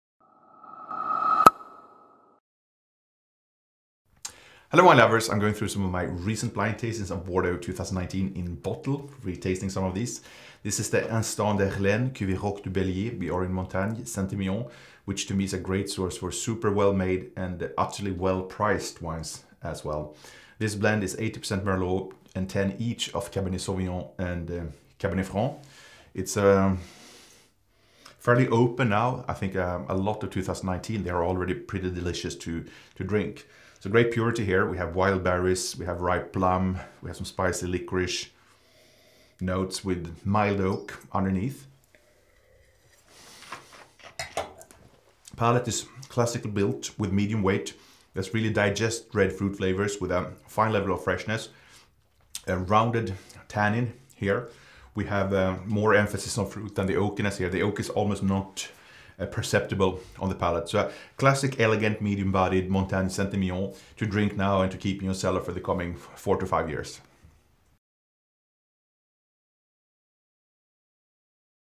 Instant d'Erlen - Cuvée Roc du Bélier 2019 - Montagne Saint Emilion
Dense and dark core with a slight purple hue. Pure nose offering wild berries, plum, liquorice and mild oak. The palate shows medium weight with digest red fruit flavours a fine level of freshness, rounded extract and rather long finish.